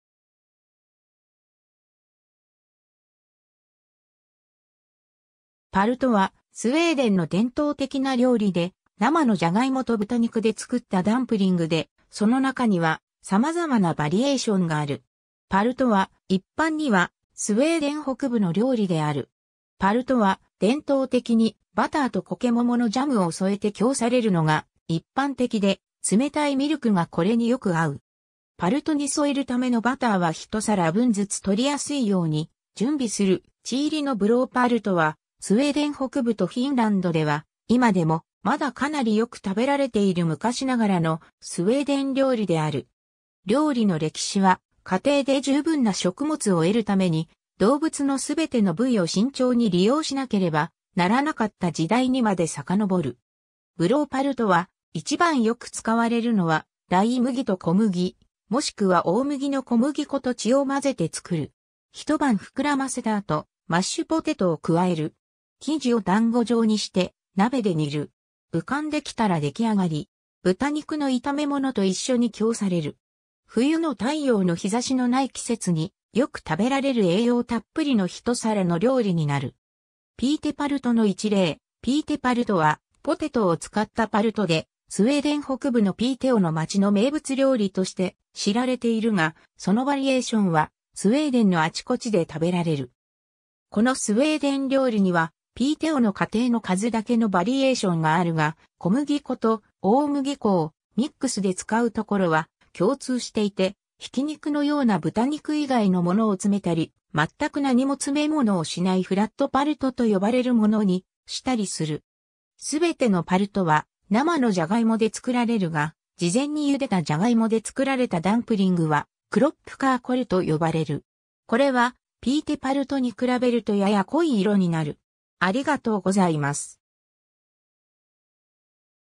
パルト
パルト(palt)は、スウェーデンの伝統的な料理で生のジャガイモと豚肉で作ったダンプリング(肉団子)で、その中にはさまざまなバリエーションがある。
パルトは、一般にはスウェーデン北部の料理である。
パルトは、伝統的にバターとコケモモのジャムを添えて供されるのが一般的で、冷たいミルクがこれによく合う。
 パルトに添えるためのバターは一皿分ずつ取りやすいように準備する 血入りのブローパルトは、スウェーデン北部とフィンランドでは今でもまだかなりよく食べられている昔ながらのスウェーデン料理である。
料理の歴史は、家庭で十分な食物を得るために、動物のすべての部位を慎重に利用しなければならなかった時代にまで遡る。
 ブローパルトは、一番よく使われるのはライ麦と小麦、もしくは大麦の小麦粉と(南では牛または豚、最北ではトナカイの)血を混ぜて作る。
 一晩膨らませた後、マッシュポテトを加える。
「生地」を団子状にして、鍋で煮る。
浮かんできたら出来上がり。
豚肉の炒め物と一緒に供される。
冬の太陽の日差しのない季節に、よく食べられる栄養たっぷりの一皿の料理になる。
 ピーテパルトの一例 ピーテパルトはポテトを使ったパルトで、スウェーデン北部のピーテオの町の名物料理として知られているが、そのバリエーションはスウェーデンのあちこちで食べられる。
 このスウェーデン料理には、ピーテオの家庭の数だけのバリエーションがあるが、小麦粉と大麦粉をミックスで使うところは共通していて、（一方、ポテトパルトのバリエーションでは、大麦粉の代わりにライ麦などの他のものを使ったり、大麦を単に外したりする）ひき肉のような豚肉以外のものを詰めたり、まったくなにも詰め物をしないフラットパルトと呼ばれるものにしたりする。
 すべてのパルトは生のジャガイモで作られるが、事前にゆでたジャガイモで作られたダンプリングは、クロップカーコル(塩漬けの豚肉と玉ねぎを弾力性のあるポテト生地で包み込んだもの)と呼ばれる。
これは、ピーテパルトに比べるとやや濃い色になる。